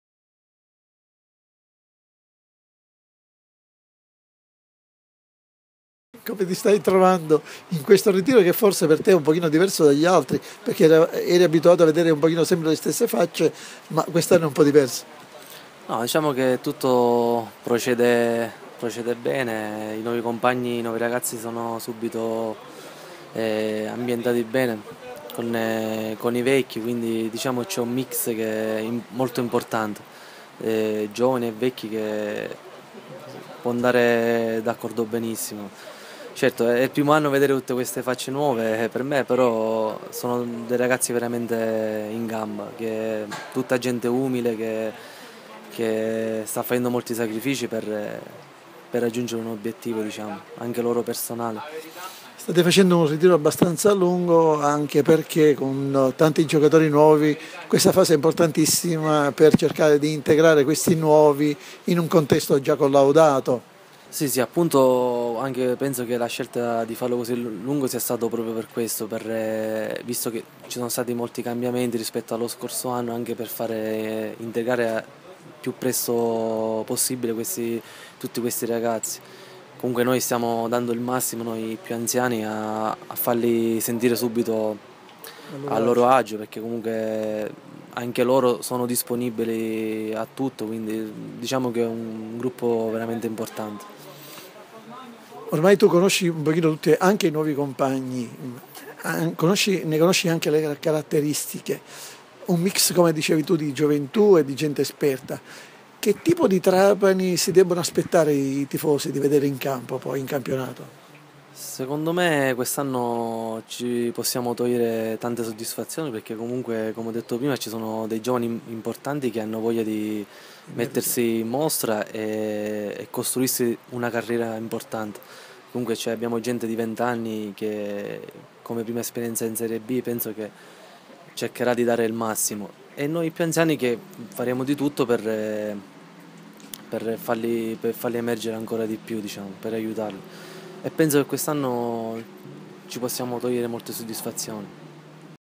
Intervista a Nino Daì - Ritiro Tarvisio ©TrapaniCalcio.it
Intervista, dal ritiro di Tarvisio del Trapani Calcio, al difensore granata Nino Daì. A cura dell'Ufficio Stampa ©TrapaniCalcio.it